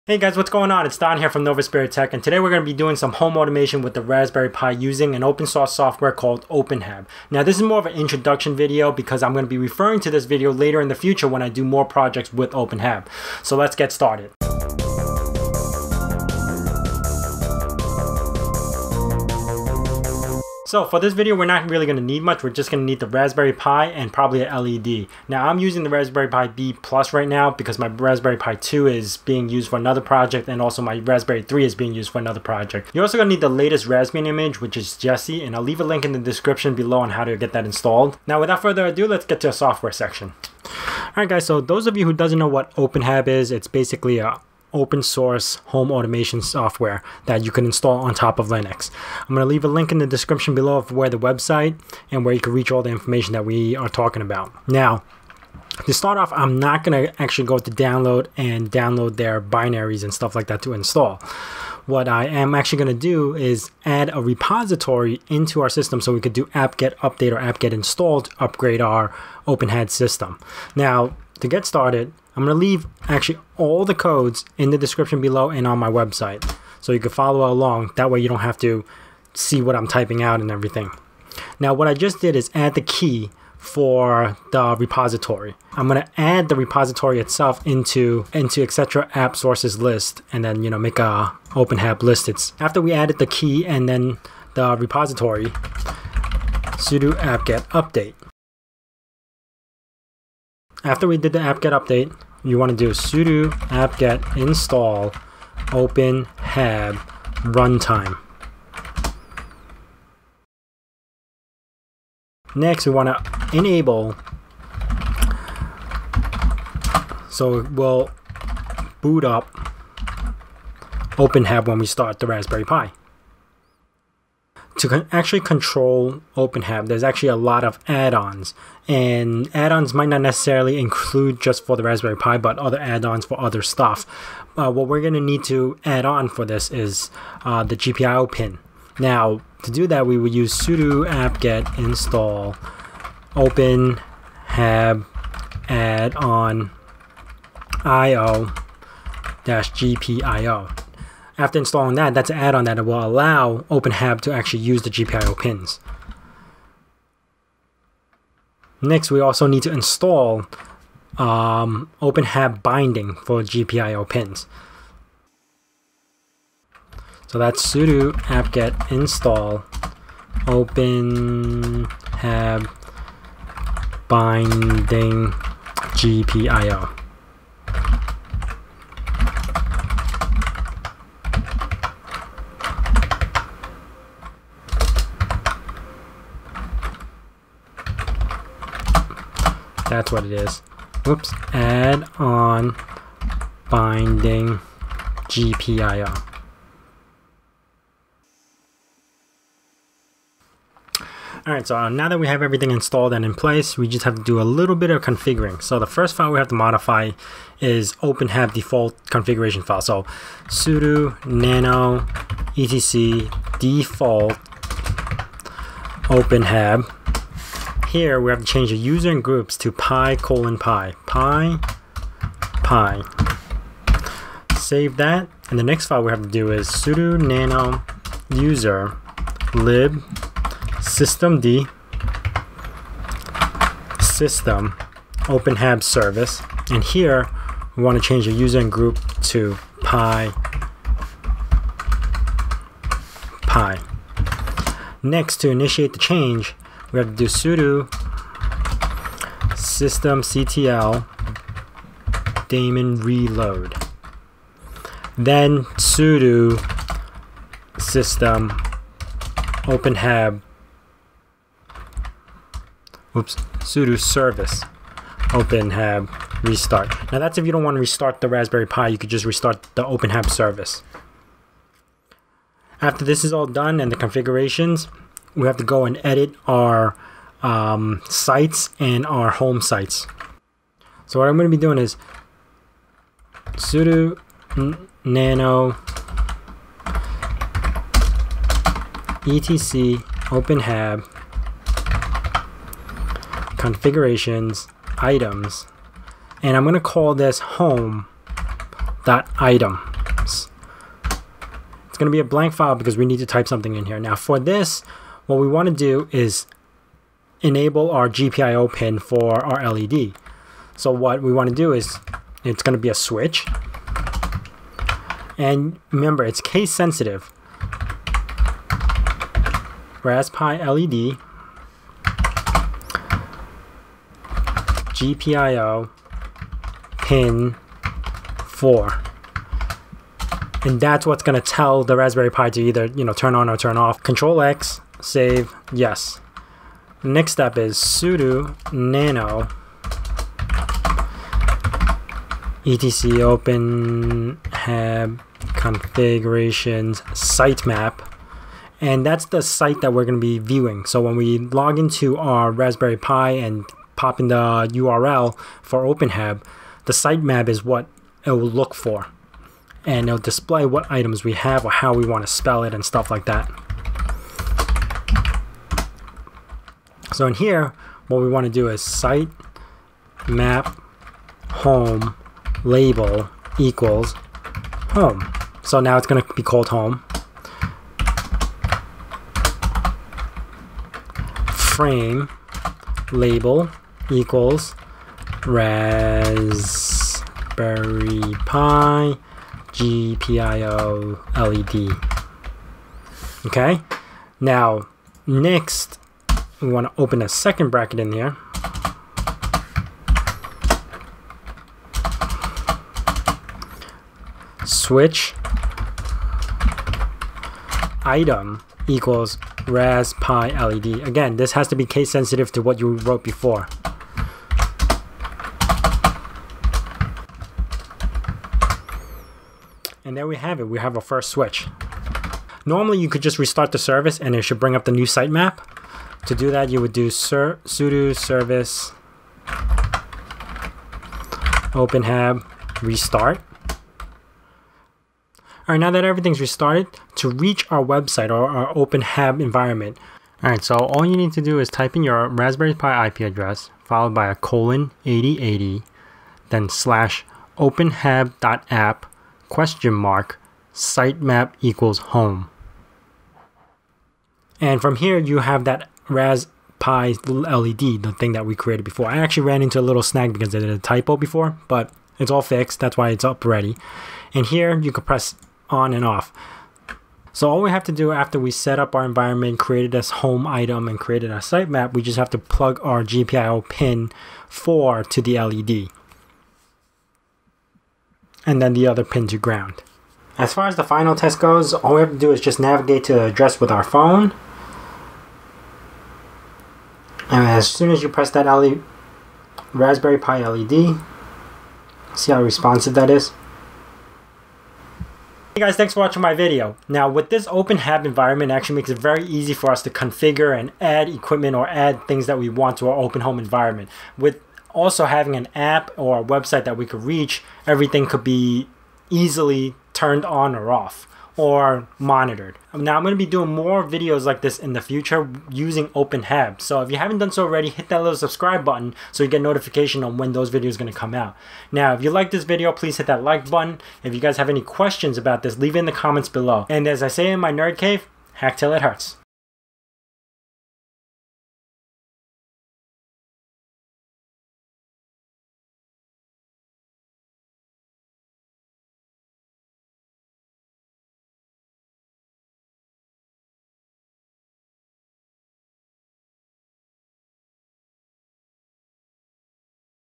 Installing OpenHab Home Automation On Raspberry Pi
This is a basic introduction on how to setup openhab on raspberry pi, i will be doing a quick blinky tutorial and getting everything setup for rpi. thanks for the view!

○○○ Quick Instructions ○○○

wget -qO - ' | sudo apt-key add

echo "deb stable main" | sudo tee /etc/apt/sources.list.d/openhab.list

sudo apt-get install openhab-runtime

sudo systemctl enable openhab

sudo apt-get install openhab-addon-io-gpio

sudo apt-get install openhab-binding-addon-gpio

sudo nano /etc/default/openhab
change openhab:openhab to pi:pi

sudo nano /usr/lib/systemd/system/openhab.service
change user to pi and change group to pi

sudo service openhab restart

// folder locations locations

service configuration /etc/default/openhab
site configuration /etc/openhab
log files /var/log/openhab
userdata like rrd4j databases /var/lib/openhab
openHAB engine, addons and /webapps folder /usr/share/openhab

// end of folder locations

** /etc/openhab/configuration/items/home.items

Switch RaspiLED{ gpio="pin:4" }

** /etc/openhab/configuration/sitemap/home.sitemap

○○○ LINKS ○○○
Raspberry Pi 2: